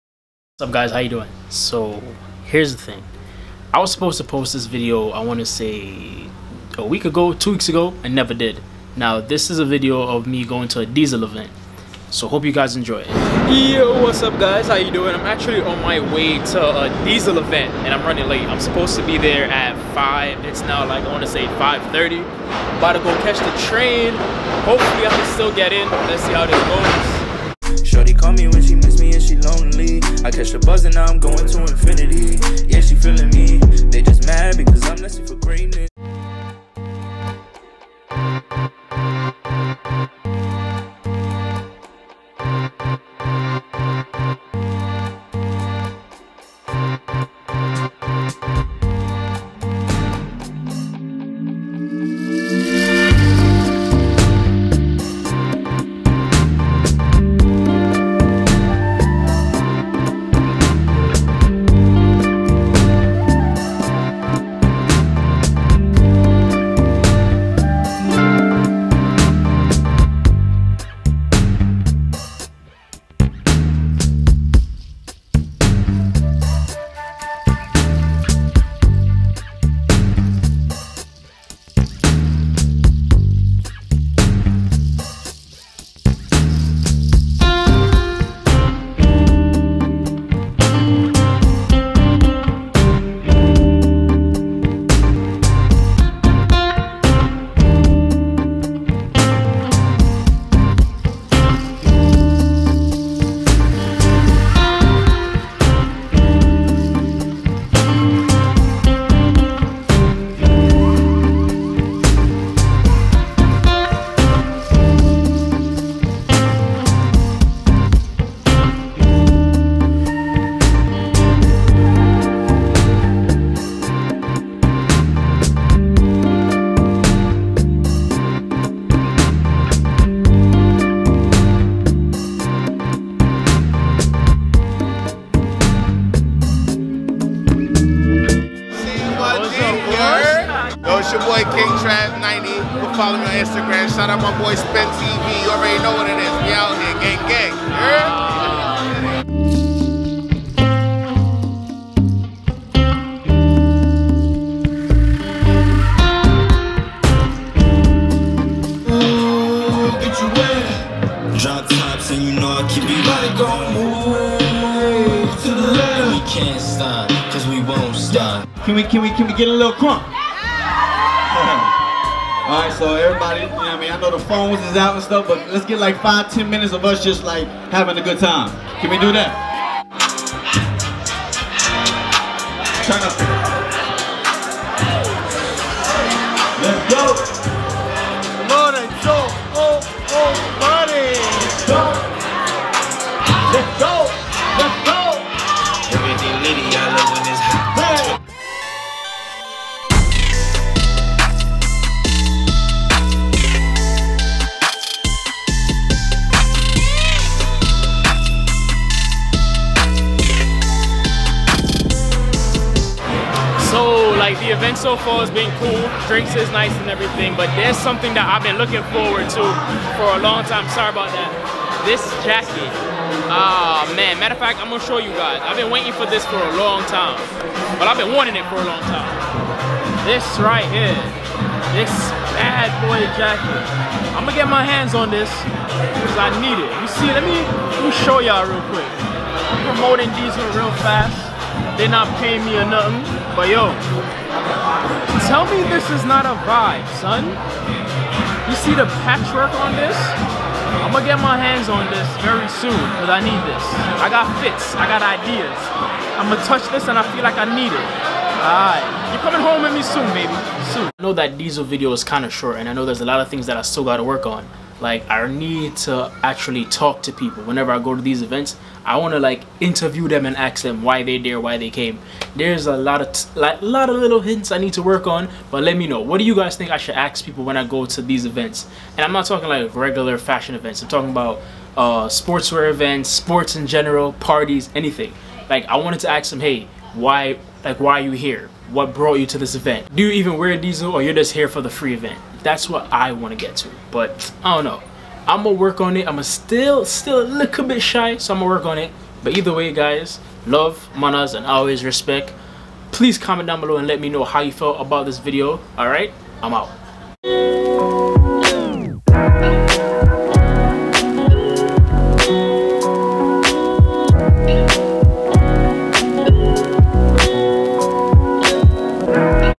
Diesel NYC x Coca-Cola Event ft Quincy
Diesel Links With Coca-Cola on a Recycled Apparel Collection. This was the release party in NYC. The (Re)Collection, a 12-piece apparel capsule that makes use of recycled PET and cotton. Which display both brands trying to save the work